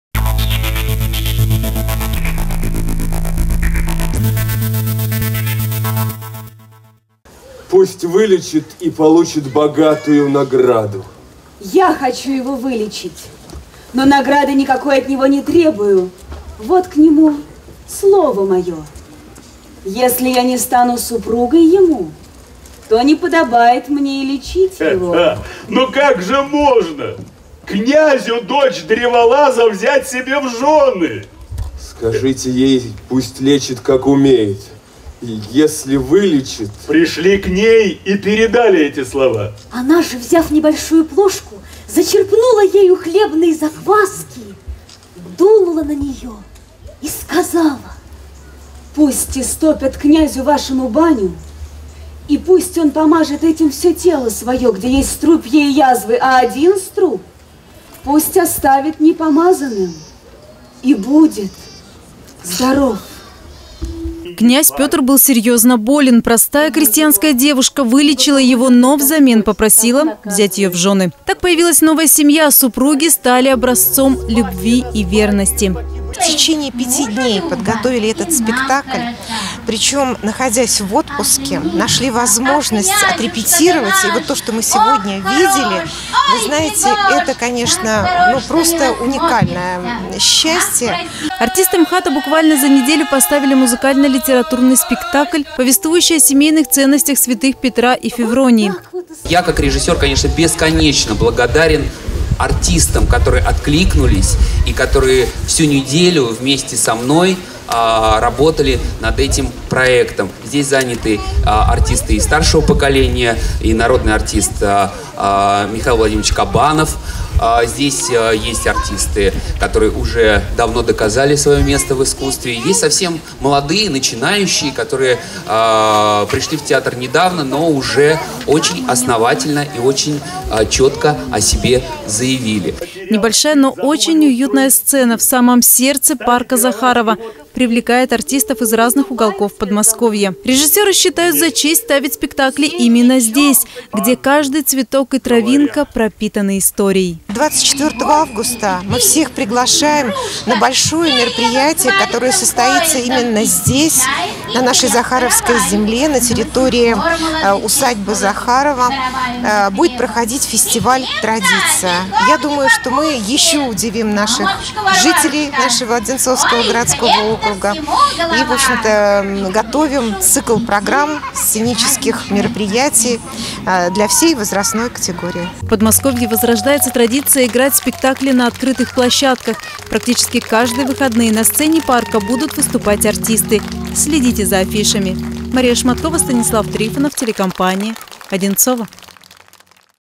День семьи, любви и верности в Захарово.
В канун Дня семьи, любви и верности в Усадьбе Голицыных сразу четыре пары официально оформили свои отношения. С церемонией бракосочетания молодожёнов поздравил и глава Одинцовского округа Андрей Иванов.